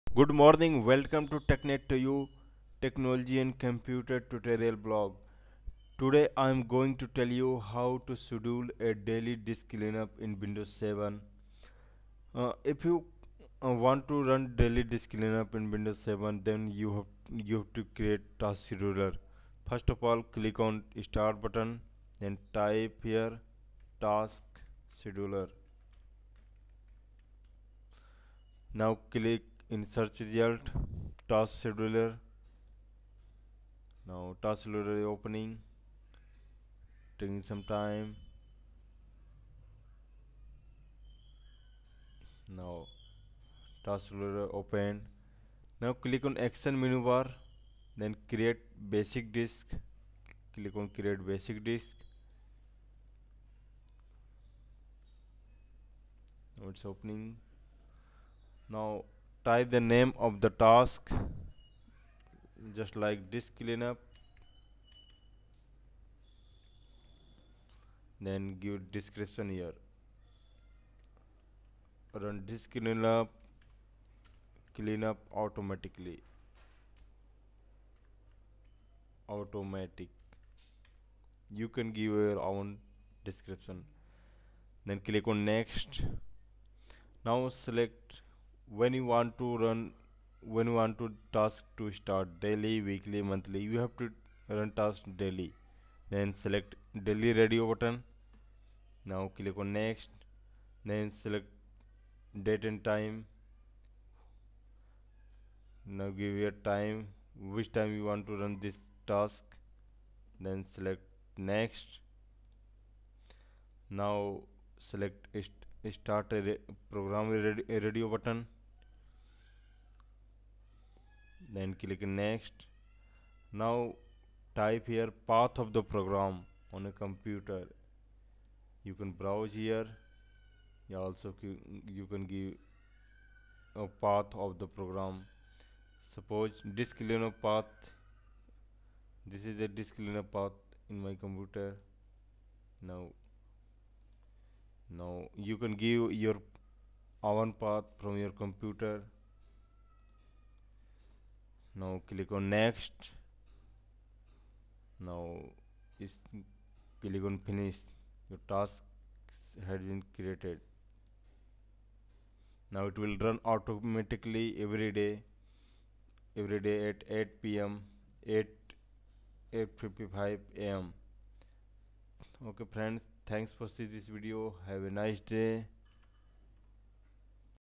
Schedule a Daily Disk Cleanup Automatic In Windows 7
technetguide You can Run Disk Cleanup daily Automatically in windows 7. Disk Cleanup Delete Temp Files and Unnecessary file from Computer. You have to create Schedule to Run Disk cleanup Daily with the Help of Task Scheduler